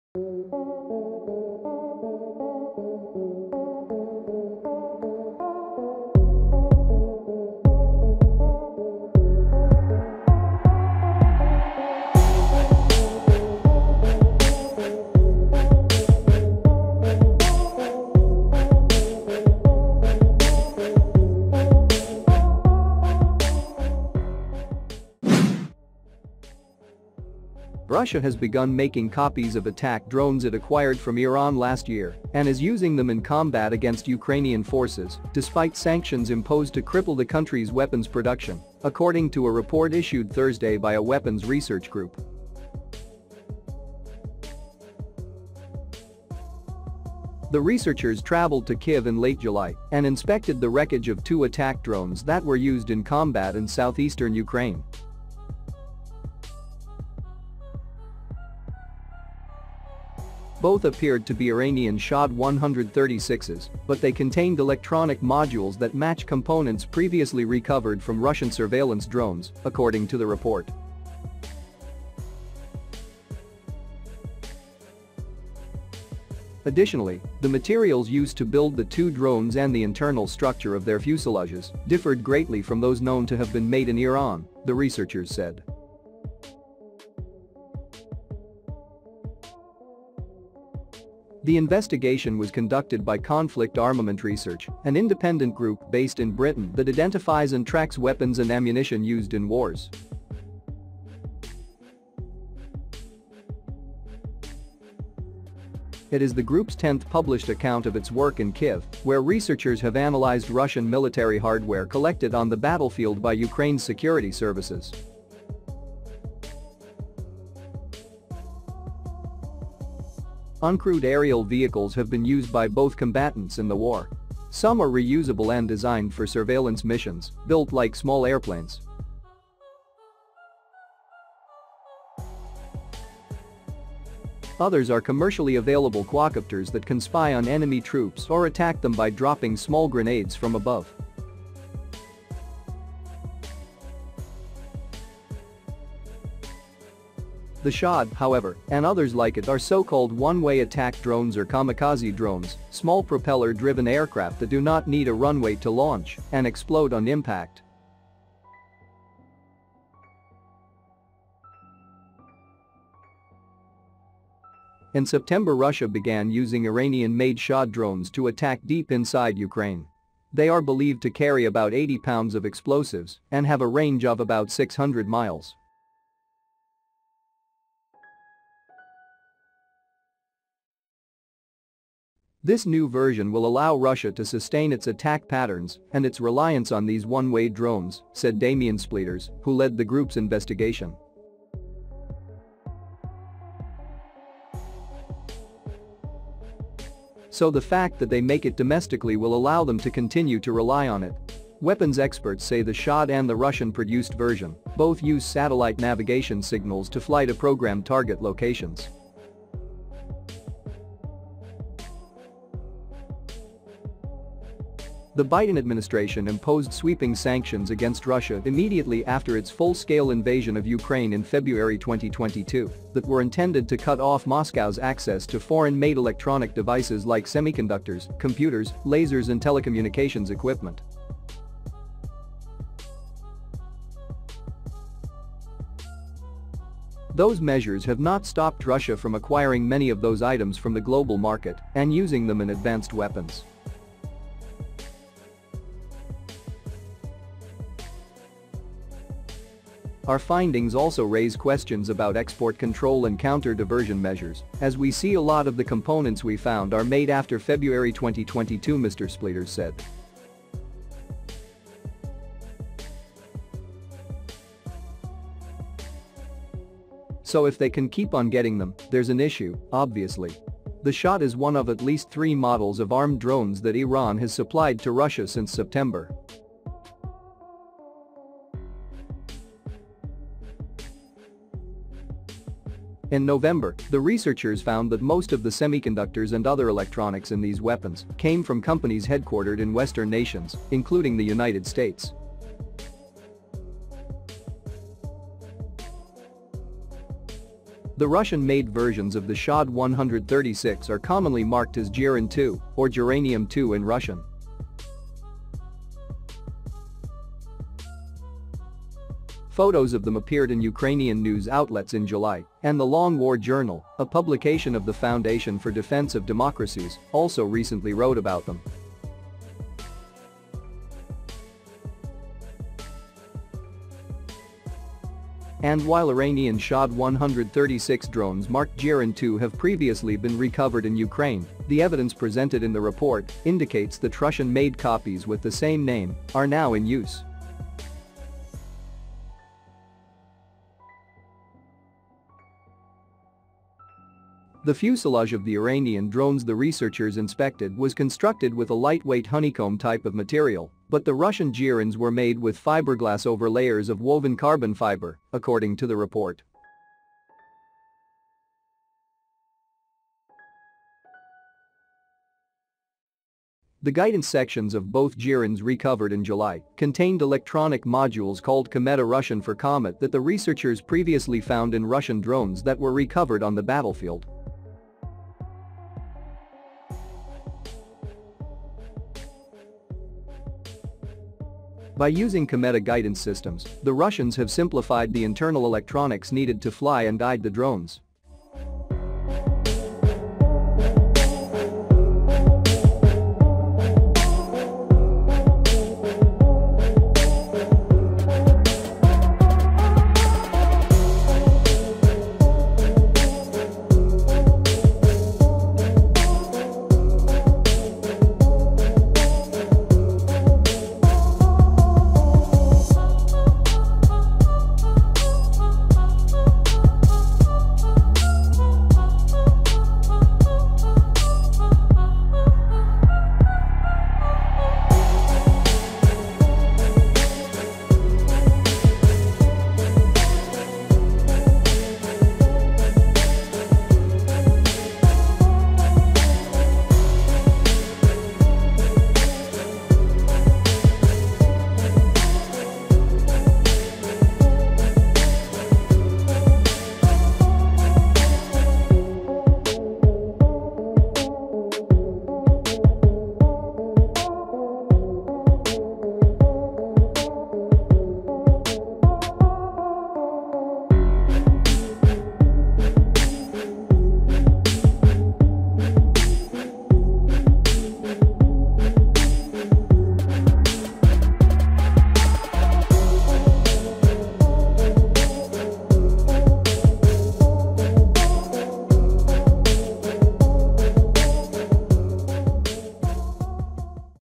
Revealed! The Reason Why Russia Still Replicating Iranian Drones and Using Them for War
Russia has begun making copies of attack drones it acquired from Iran last year and is using them in combat against Ukrainian forces despite sanctions imposed to cripple the country’s weapons production, according to a report issued Thursday by a weapons research group.